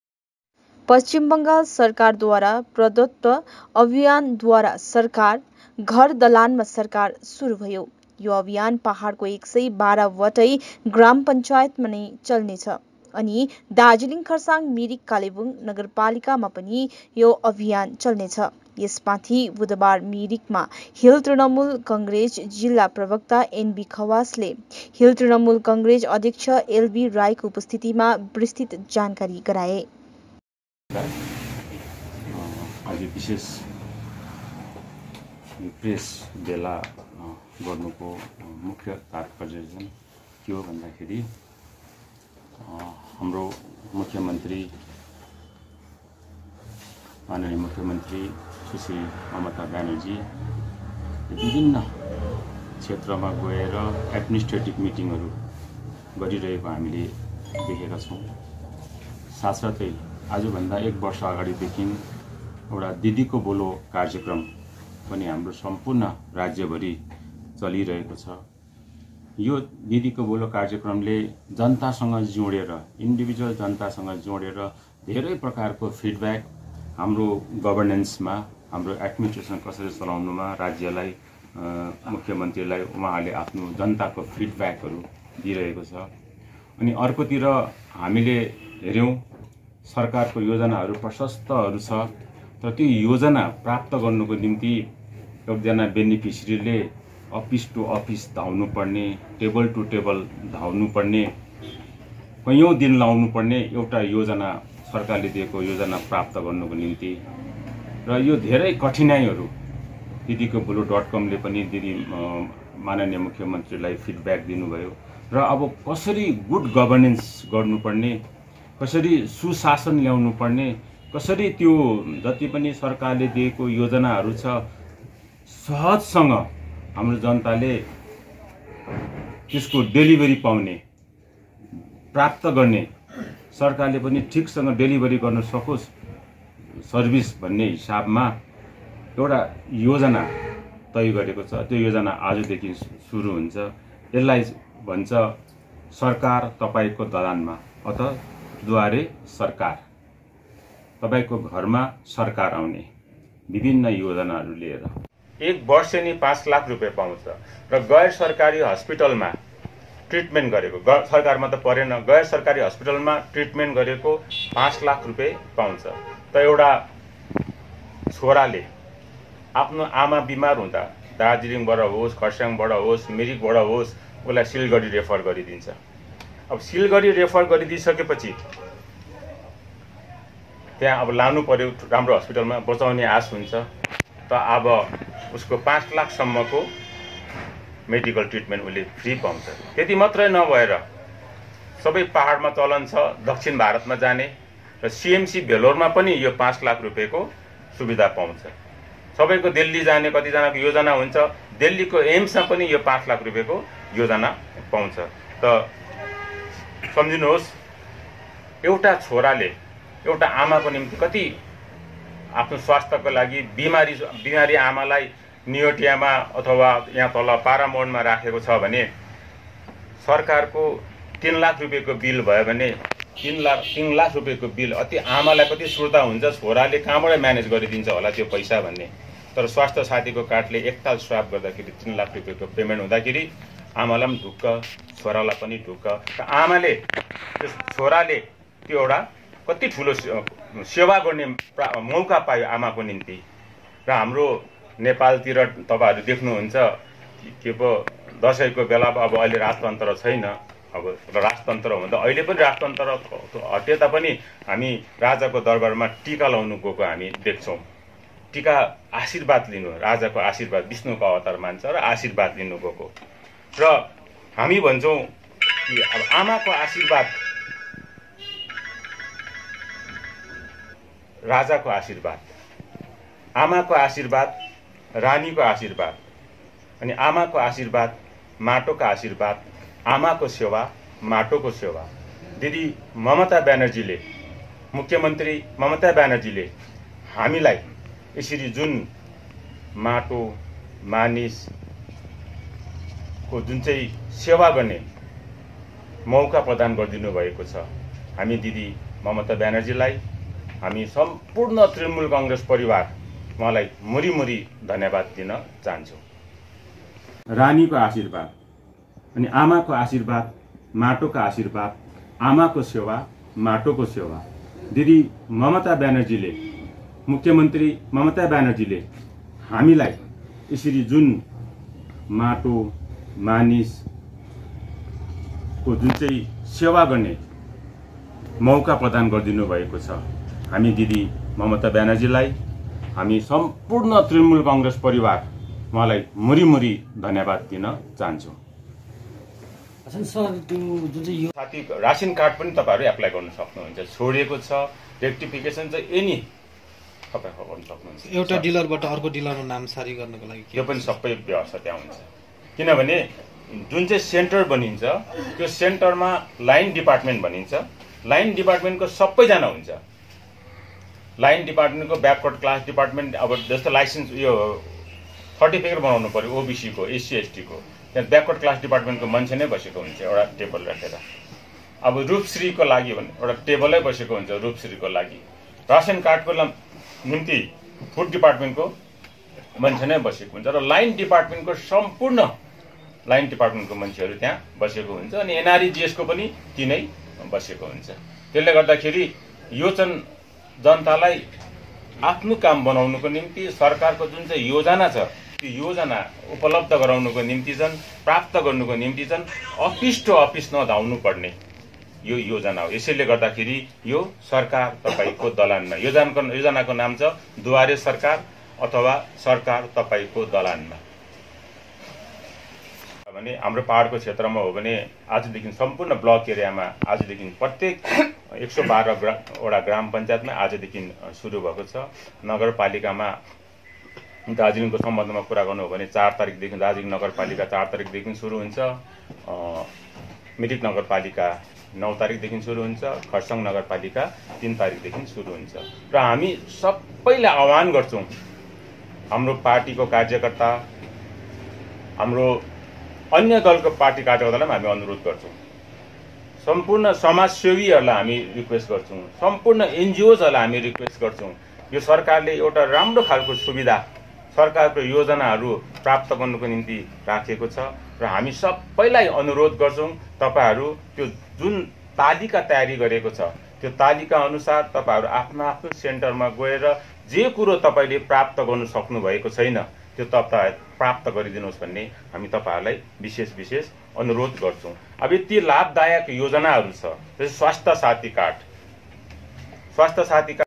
Darjeeling news पश्चिम बङ्गाल सरकारद्वारा प्रदत्त अभियान द्वारे सरकार ( घर दलानमा सरकार ) शुरु भयो ।
पश्चिम बङ्गाल सरकारद्वारा प्रदत्त अभियान द्वारे सरकार ( घर दलानमा सरकार ) शुरु भयो ।